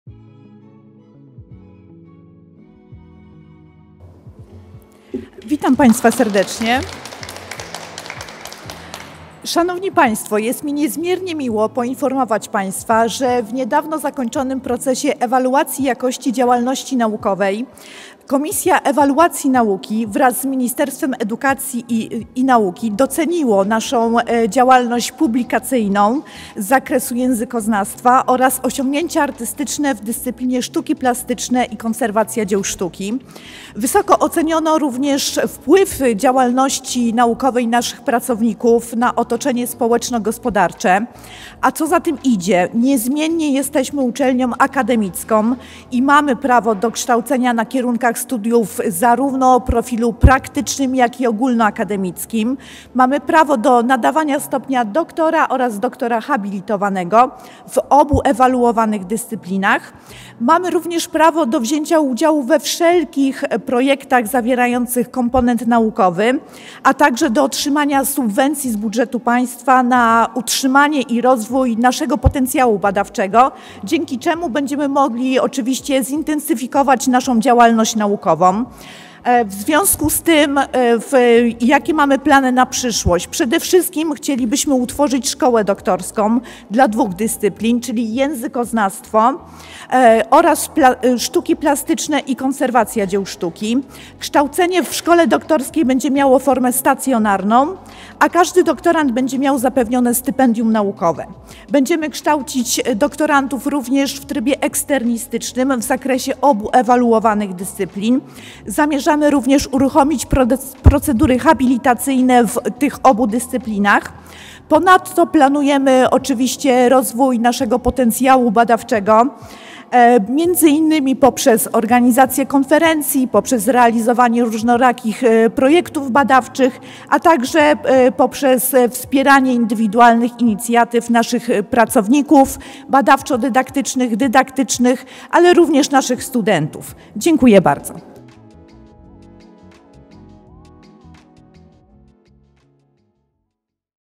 Szkoła doktorska, projekty badawcze w Akademii
Jakie są plany Akademii w związku z zakończonym procesem ewaluacji działalności naukowej? Mówi o tym dr Marzena Fornal, prorektor ds. nauki.